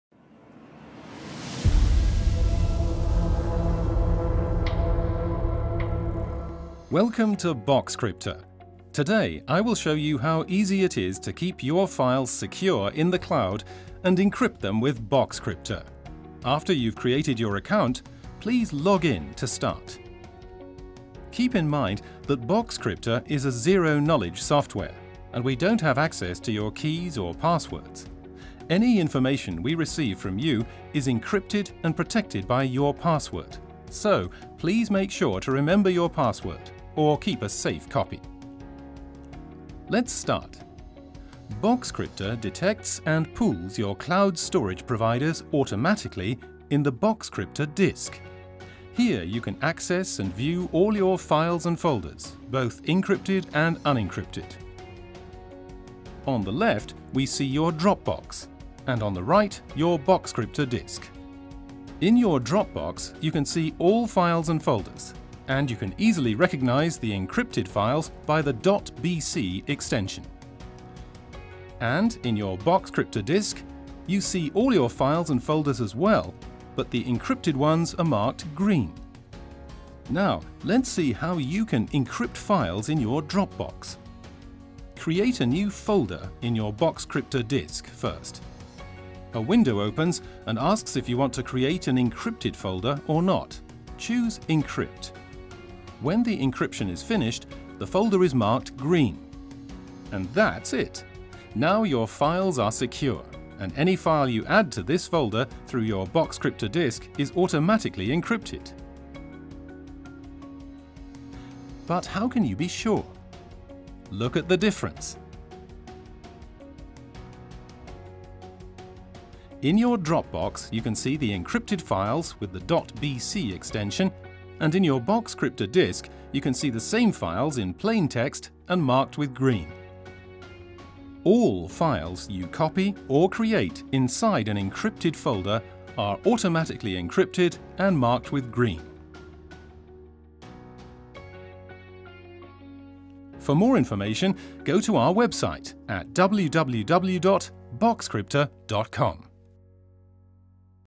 Boxcryptor for macOS | How it works
𝗘𝗻𝗰𝗿𝘆𝗽𝘁𝗶𝗼𝗻 “𝗠𝗮𝗱𝗲 𝗶𝗻 𝗚𝗲𝗿𝗺𝗮𝗻𝘆” – 𝗛𝗶𝗴𝗵𝗲𝘀𝘁 𝗦𝗲𝗰𝘂𝗿𝗶𝘁𝘆 𝗳𝗼𝗿 𝘆𝗼𝘂𝗿 𝗙𝗶𝗹𝗲𝘀 𝗶𝗻 𝘁𝗵𝗲 𝗖𝗹𝗼𝘂𝗱
Boxcryptor for macOSprovides a user-friendly, additional layer of security for cloud storages by encrypting files locally on your device. Since Boxcryptor was optimized for the cloud from the very beginning, the encryption takes place on every file and access can be shared. This means that every file is encrypted independently from the others. Additionally, typical cloud storage features, such as file history or selective sync, are supported.

- For macOS 10.14 or later
- Free for personal use
- Seamless workflow with integration in Apple’s Finder
- Auto detection of cloud providers
- Set file and folder options via context menu